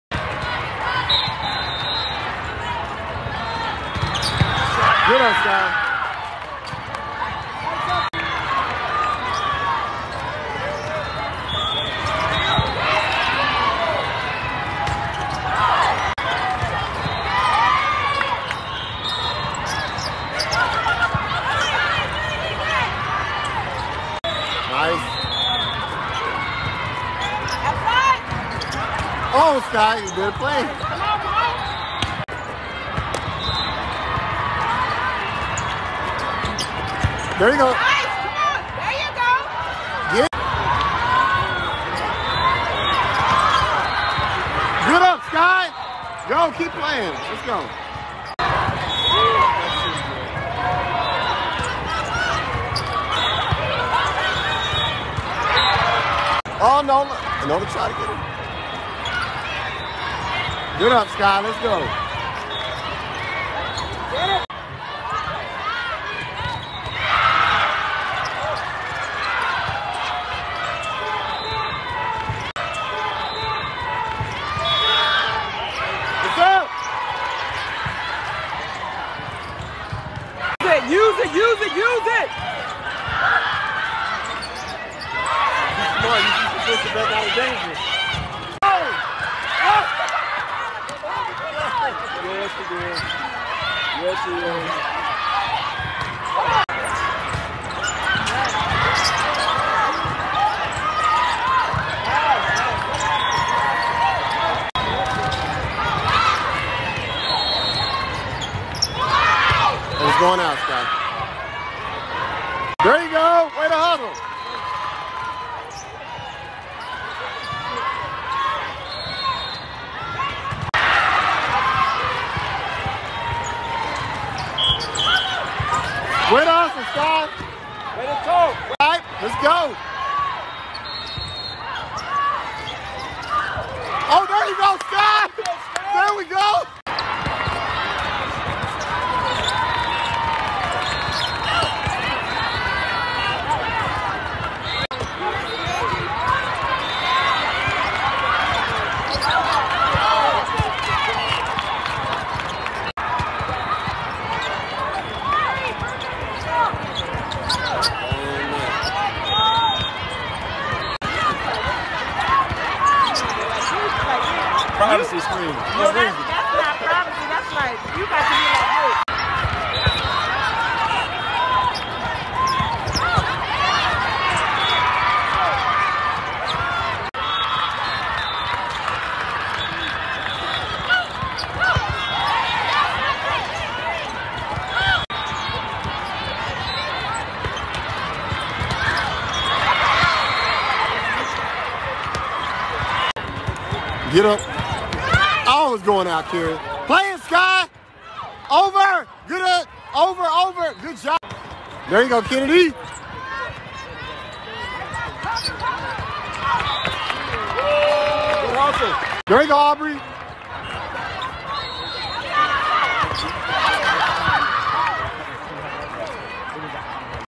Skye Graham - 27 Digs @ MEQ 2022
Skye Graham play great defense.  Spectacular digs.